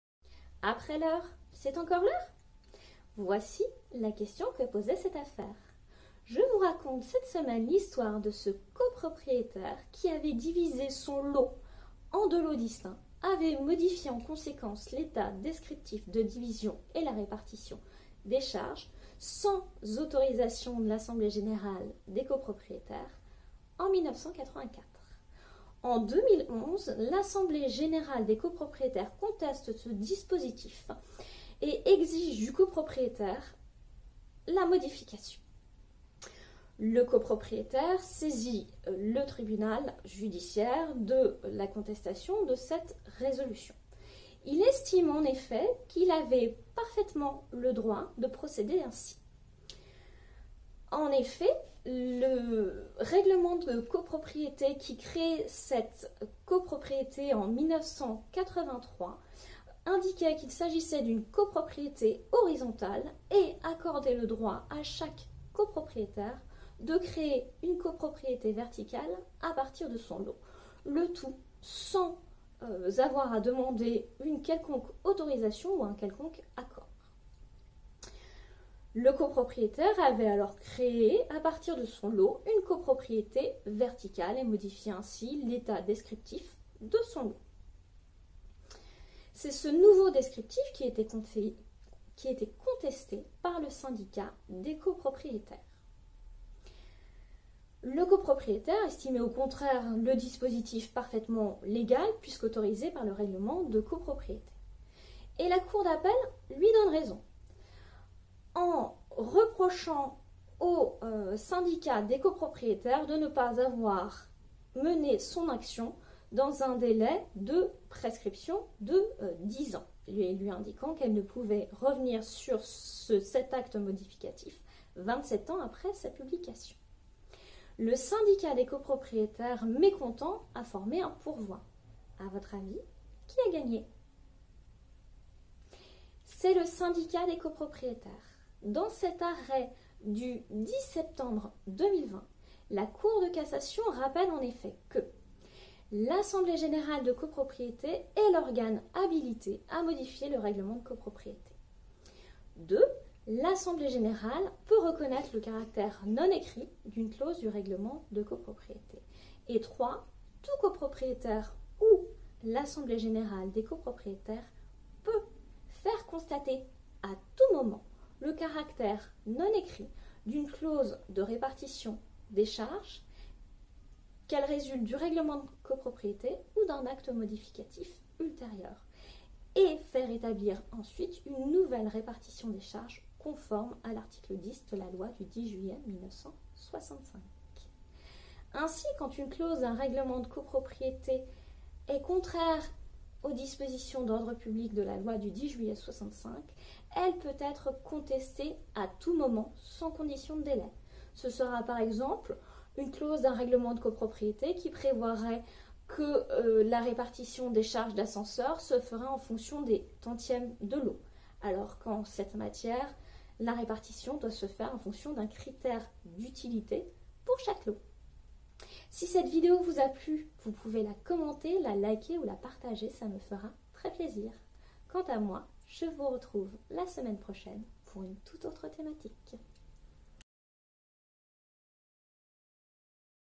l'illégalité d'une clause du règlement de copropriété
Après l’heure, c’est encore l’heure ?

Voici la question que posait cette affaire.

Je vous raconte cette semaine l’histoire de ce copropriétaire qui avait divisé son lot en deux lots distincts, modifié en conséquence l’état descriptif de division et la répartition des charges, sans autorisation du syndicat des copropriétaires en … 1984.

En 2011, l’assemblée générale des copropriétaires conteste la légalité du dispositif et exige sa modification.